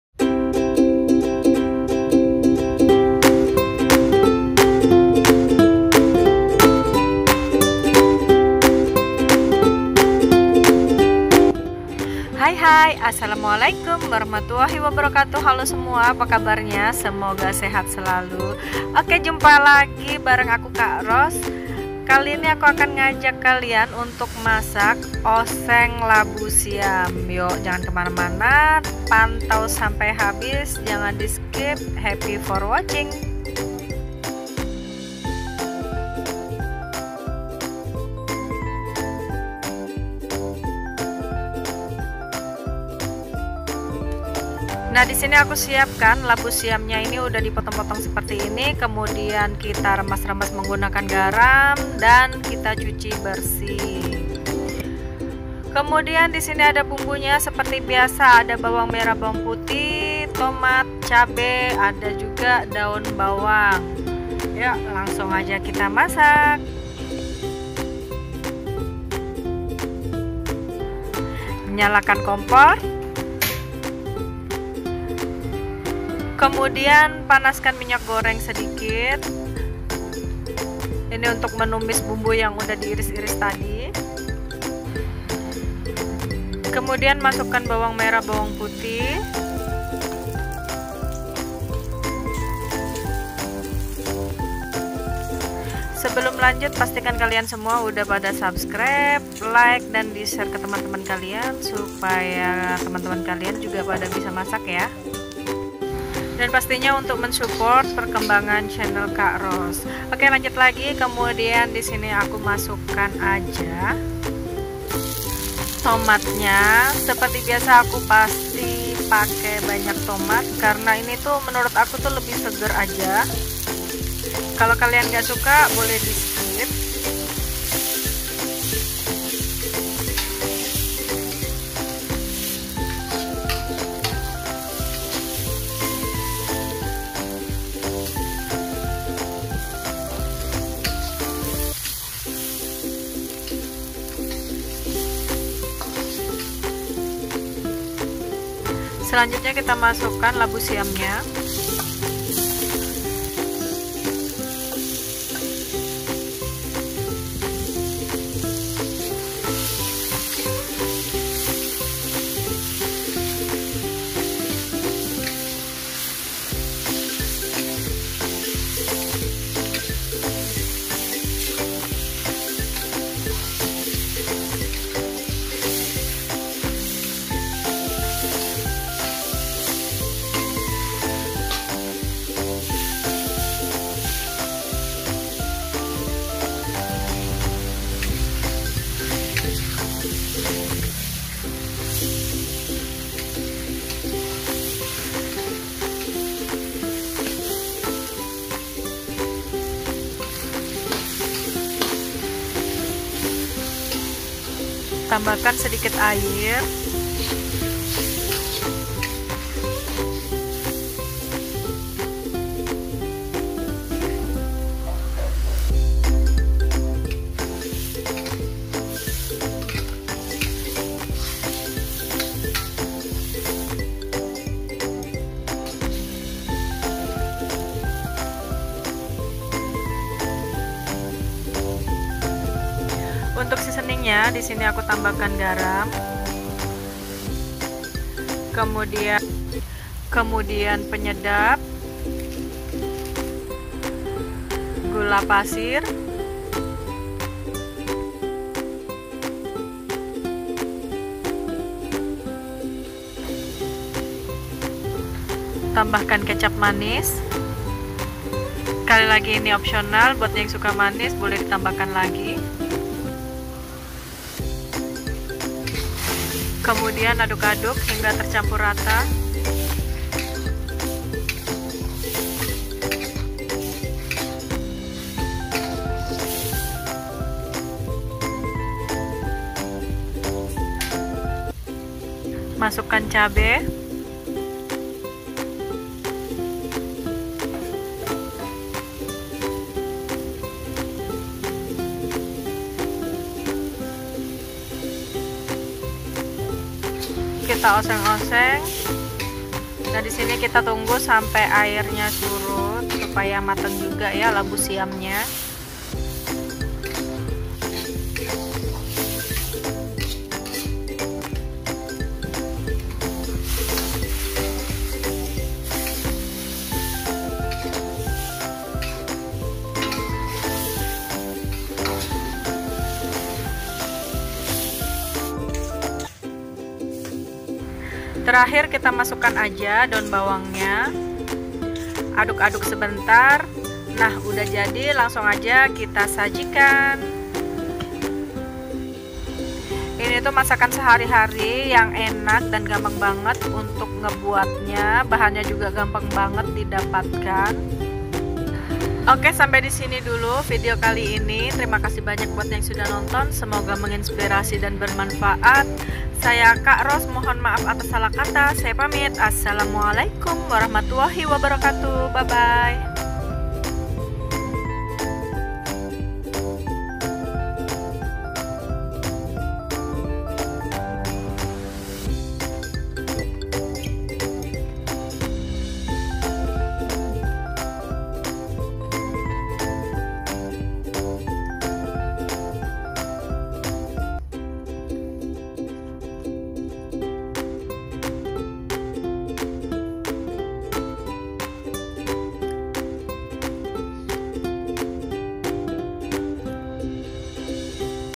RESEP TUMIS LABU SIAM SIMPEL, MUDAH DAN ENAK BANGET
TUMIS LABU SIAM

Bahan :
Labu siam
bawang merah
bawang putih
tomat
cabe
daun bawang
gula pasir
garam
penyedap
kecap manis

untuk cara membuatnya silahkan tonton vidio di atas .